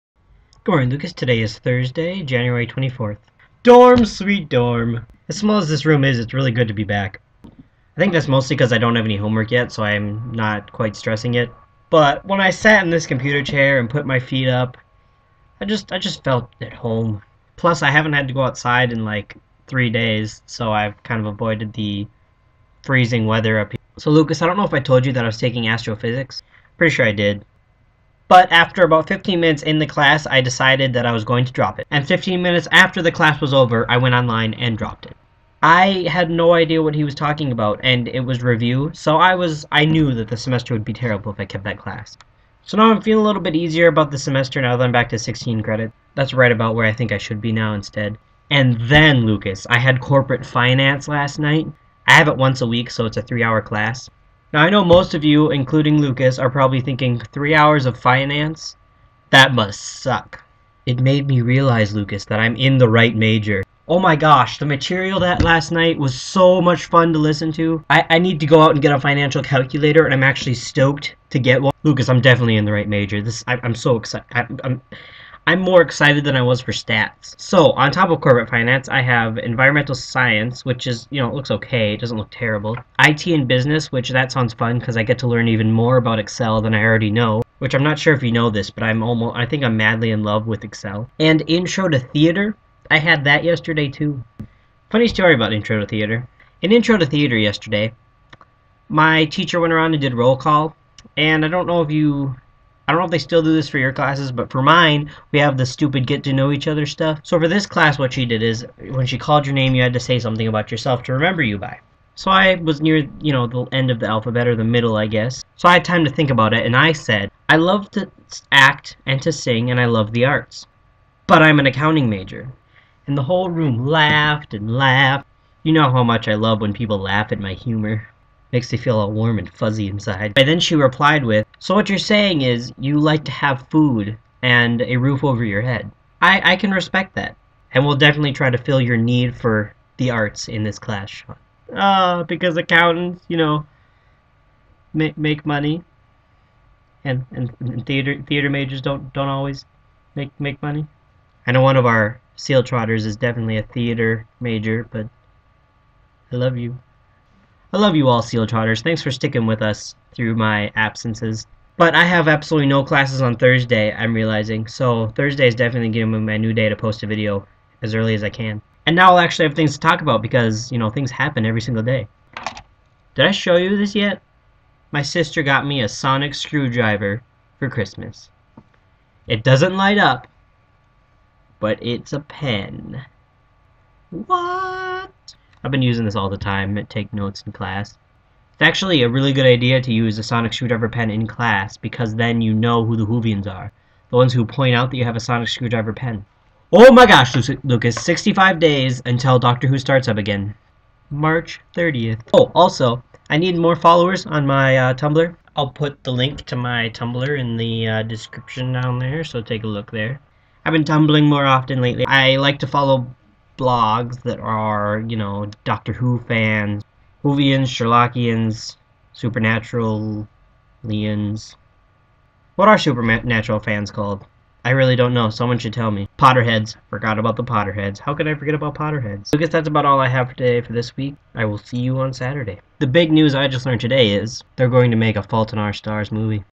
Back at College to Get More Knowledge
In which Sean rejoices for being back in his cozy dorm, discusses how his first week of classes have gone, fangirled a lot and tried to spread the word about his tumblr.

Link to the article where I discovered TFIOS was going to be a movie. achieved from tumblr: